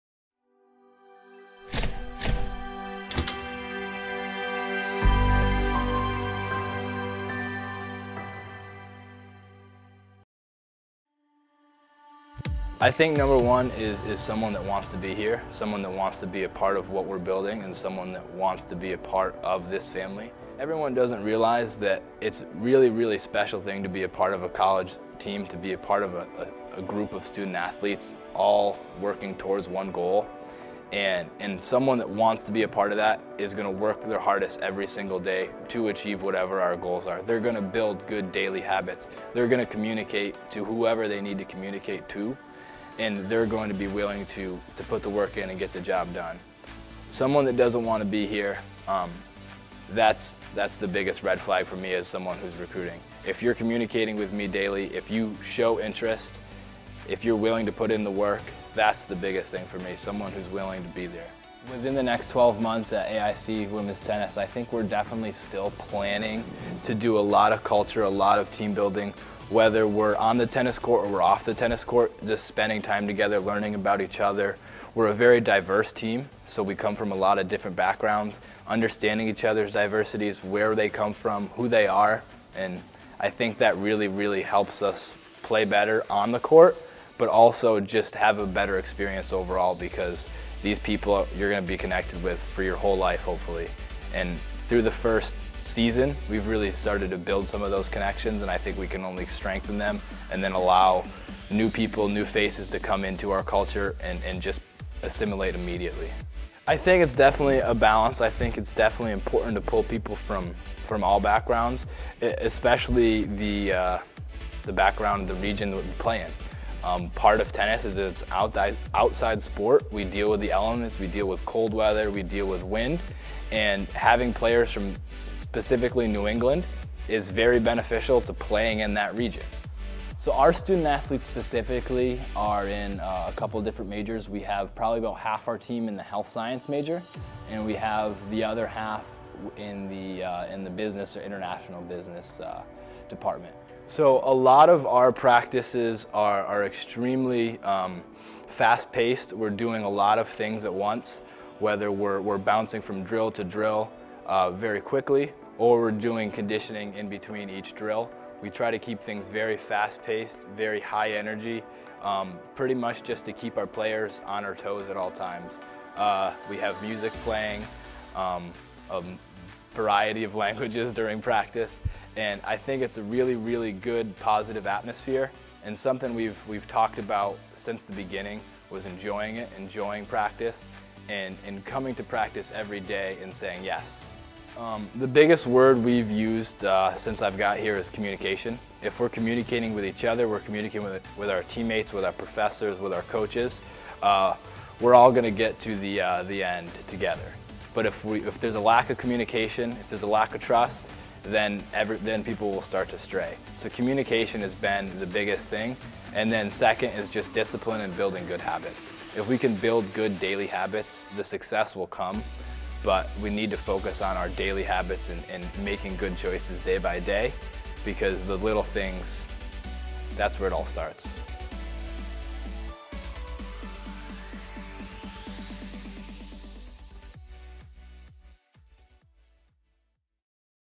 Get to know the AIC Women's Tennis Program
Head women's tennis coach Tennis DeVarennes talks about the AIC women's tennis program and what he is looking for when searching for future Yellow Jackets!

Do you want to be a Yellow Jacket? Start the process today by visiting